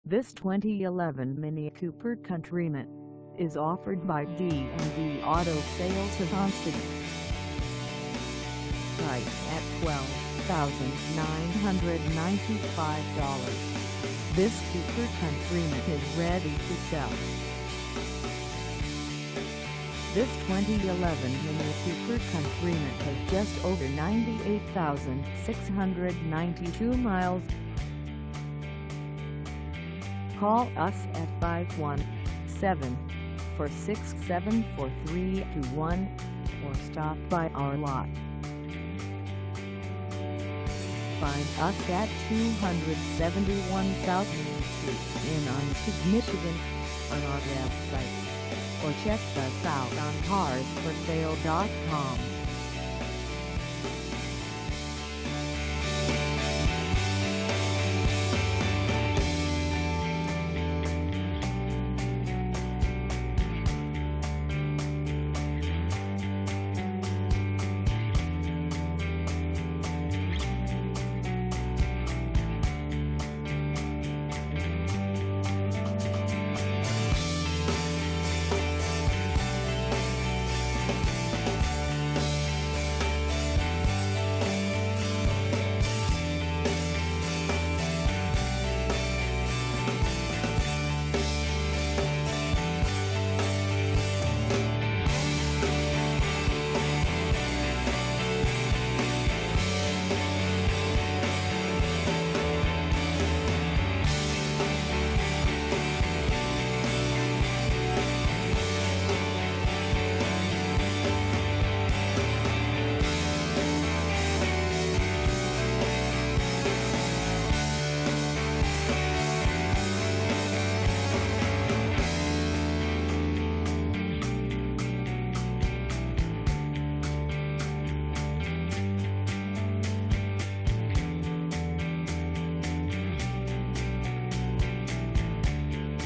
2011 MINI Cooper Countryman Base 4dr Crossover for sale in O
This 2011 MINI Cooper Countryman Base 4dr Crossover is for sale in Onsted, MI 49265 at D and D Auto Sales Of Onsted.

Engine: 1.6L I4
Transmission: Manual 6-Speed
Color: White
Mileage: 98692
Contact D and D Auto Sales Of Onsted 271 S Main St Onsted, MI 49265 to test drive this 2011 MINI Cooper Countryman Base 4dr Crossover today.